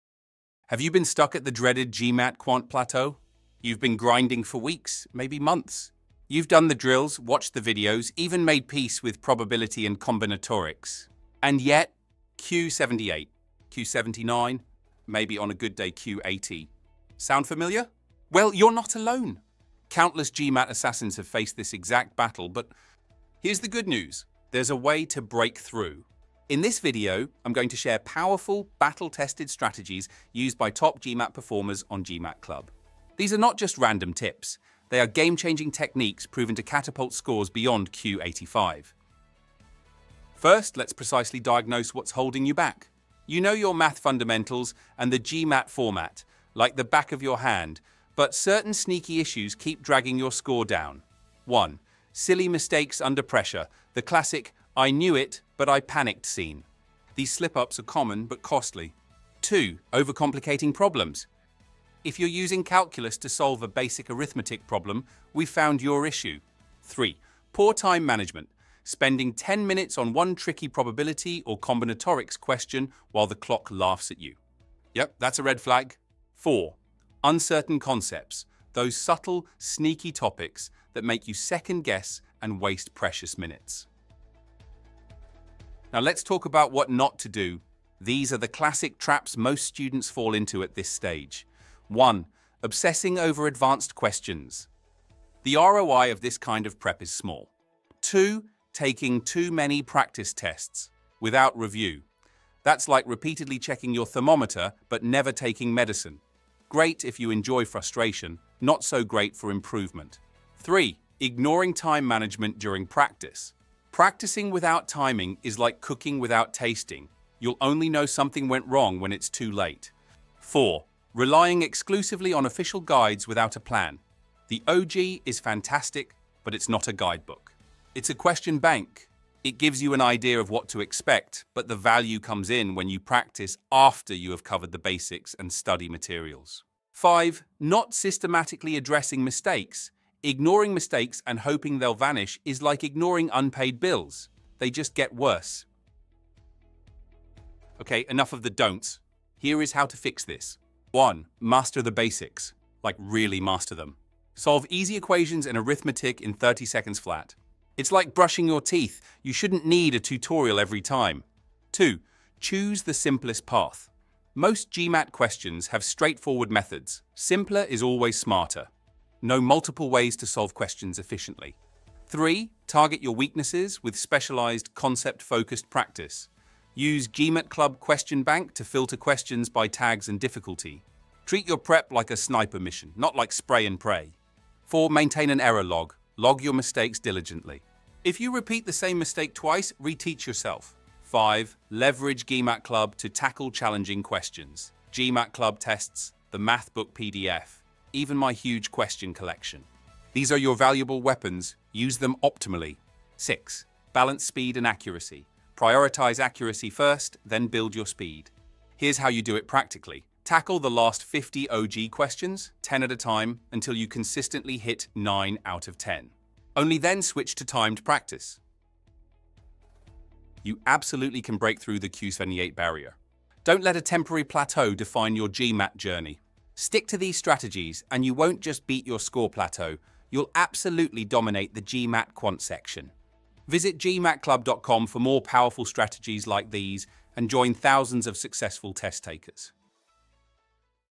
4 Reasons Why You’re Not Improving in GMAT Quant (and How to Fix It)
Are you stuck at Q78-Q80 in GMAT Quant despite relentless preparation? Many test takers plateau at this exact range. In this video we reveal the root causes of score stagnation and proven strategies that GMAT Club’s top scorers used to go from a frustrating Q78 to a dominant Q85+. You will learn how to diagnose and tackle the sneaky pitfalls holding you back: silly mistakes, overcomplicated methods, poor time management, and subtle concept gaps. Avoid common GMAT Quant mistakes and discover effective study techniques like targeted practice, precise error logging, simplified problem-solving methods, and leveraging resources like GMAT Club.

✅ Key Discussion Points:
- Diagnosis of exactly what's limiting your GMAT Quant score- Critical mistakes that keep your score plateaued- How to target your weaknesses with precision- How to utilize GMAT Club's resources effectively

📚 For more expert strategies, visit GMATclub.com and join thousands of successful test-takers.

Video Chapters:
00:00 The Dreaded Q78 Block
00:40 4 Potential Reasons for Score Stagnation
01:25 5 Things to Avoid in Preparation
02:27 Action Plan for Q78 to Q85+